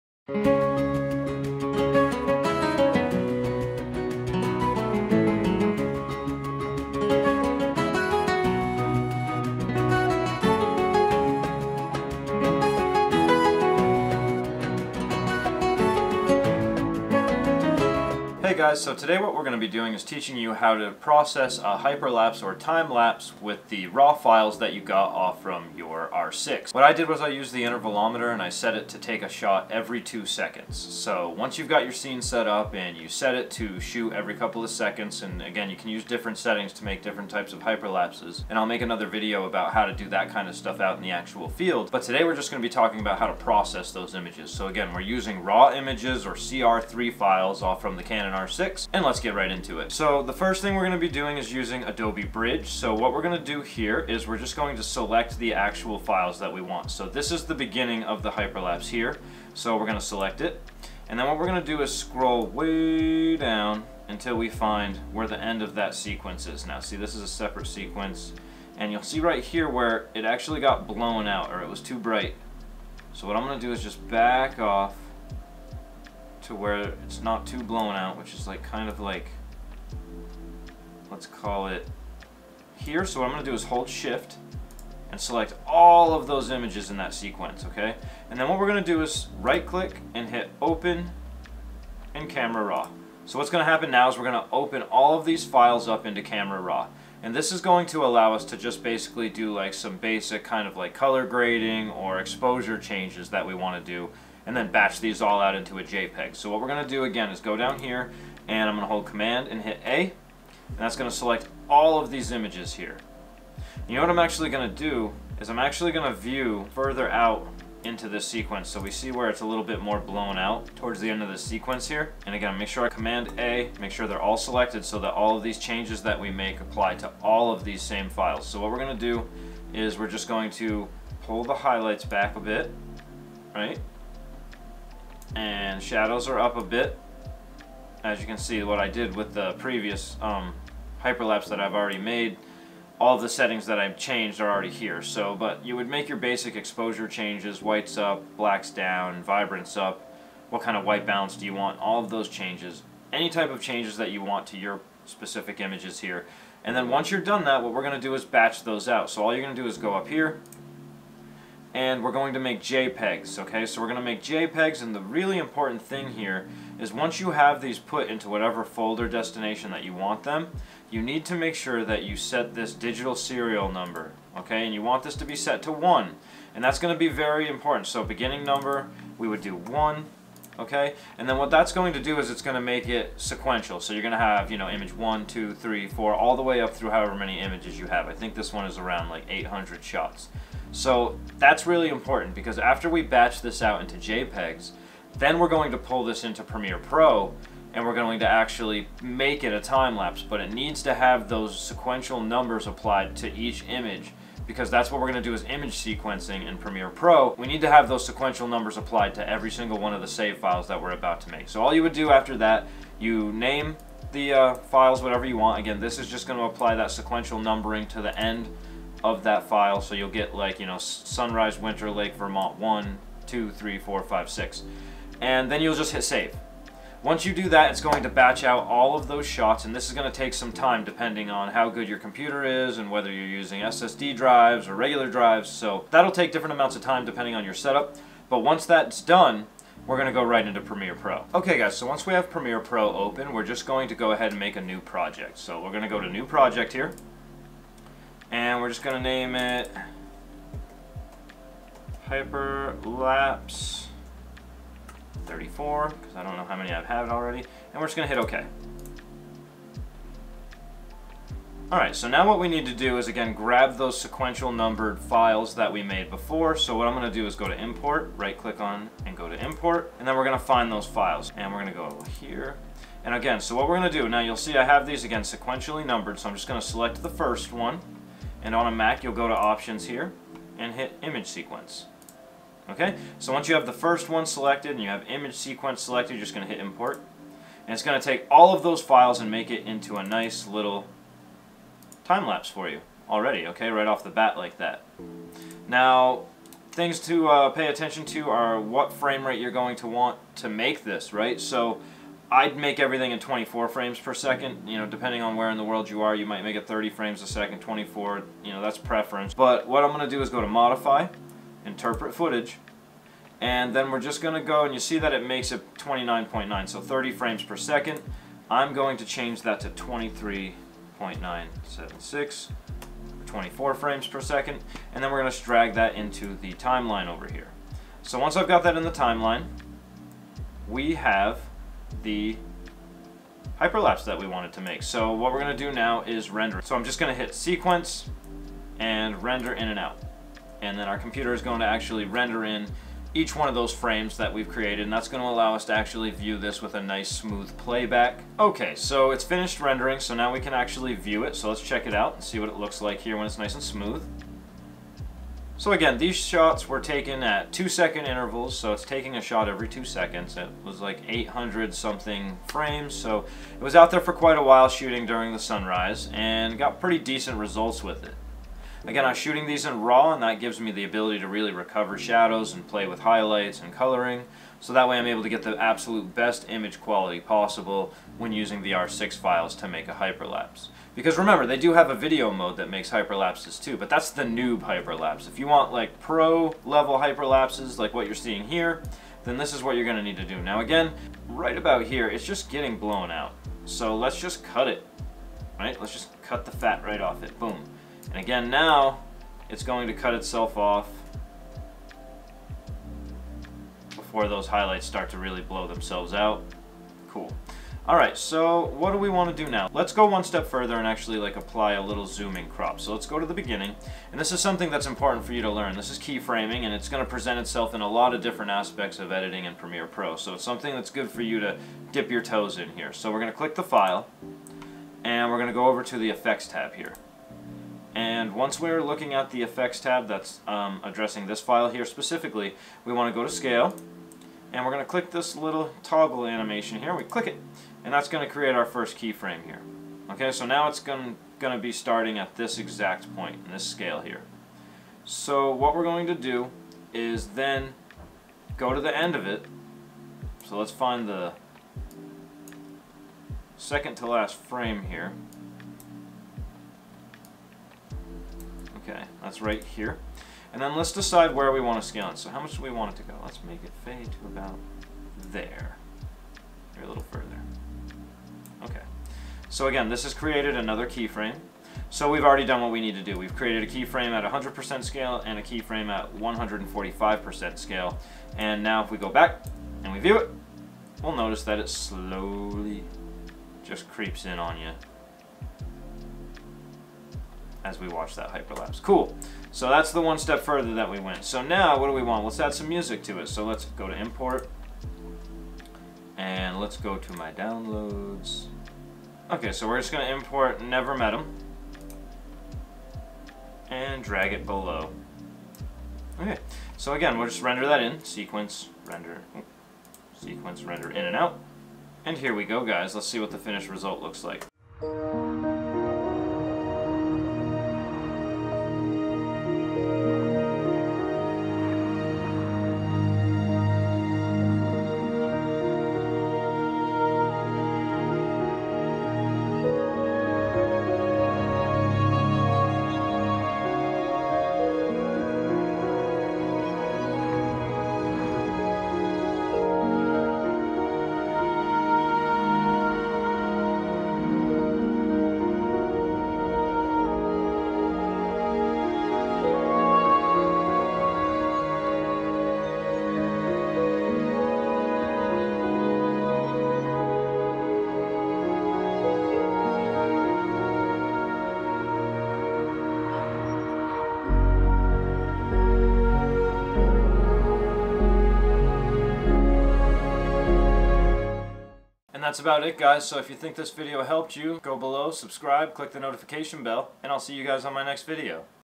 How to make a Time-lapse | Adobe Bridge and Premiere Pro | Time-lapse with RAW images | Tutorial
Find out how I make Time-lapse videos out of my .RAW files from my Canon R6 in this short tutorial video using Adobe Bridge and Premiere Pro!